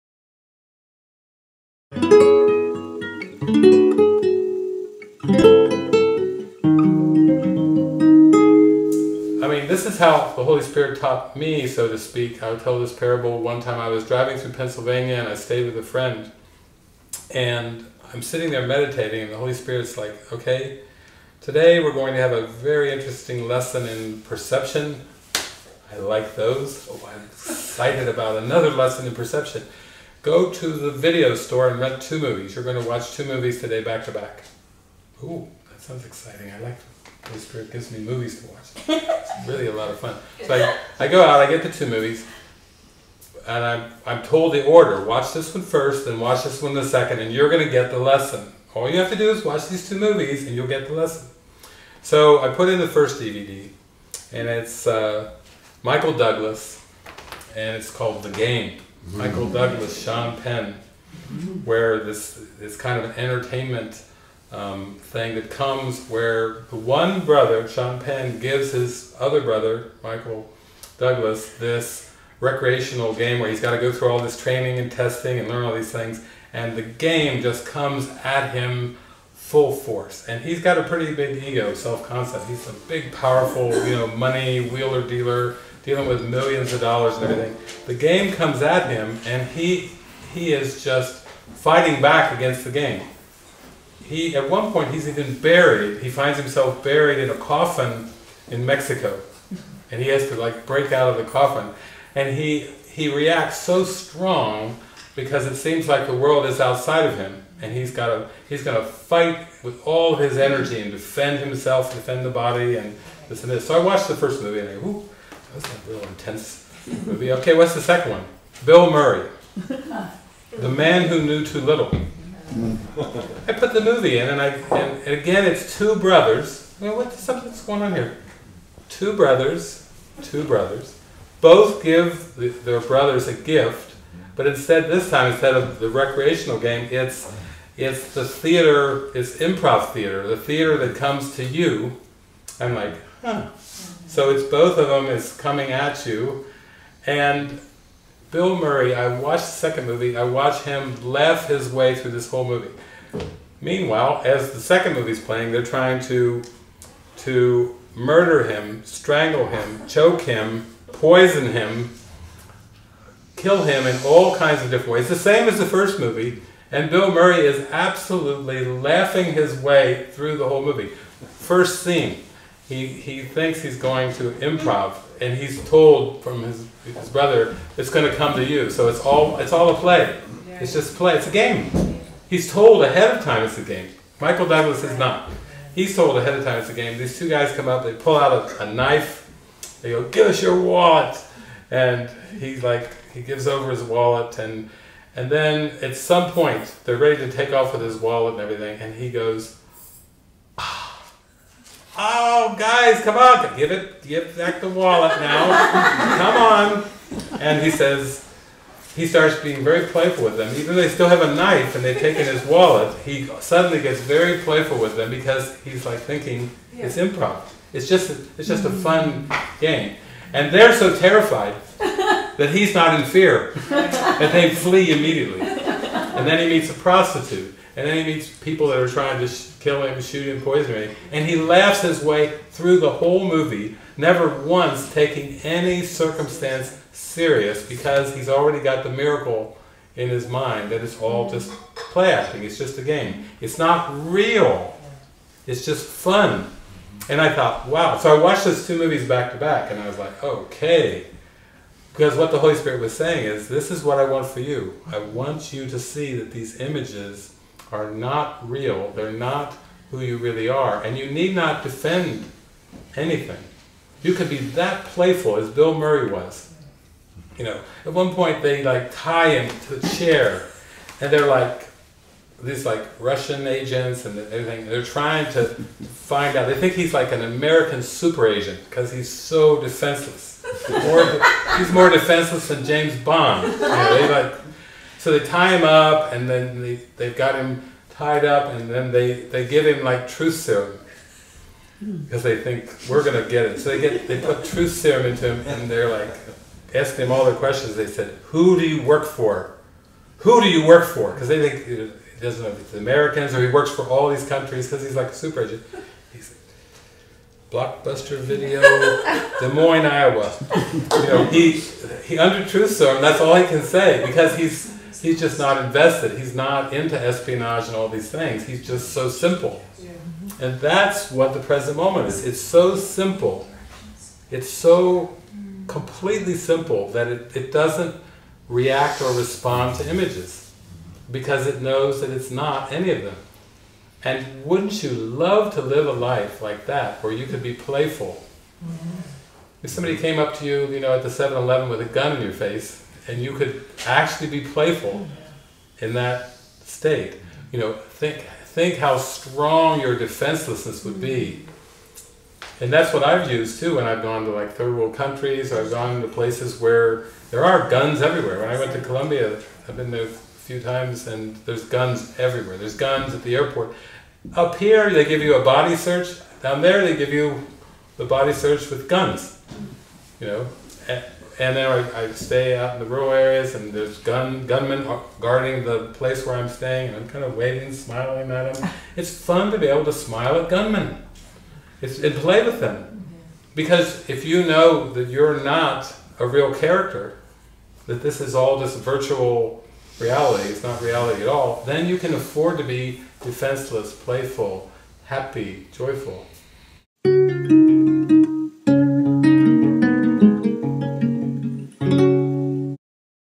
ACIM Online Teachings - Awakening With Movies David Hoffmeister A Course in Miracles
Practice and live the mystical teachings of Jesus assisted by online courses, online retreats, and an online Tribe of Christ community:
David tells the parable of the Holy Spirit teaching through movies. The movies are: The Game and The Man Who Knew Too Little.  See also The Movie Watcher's Guide to Enlightenment for more movies and insights on how to watch them.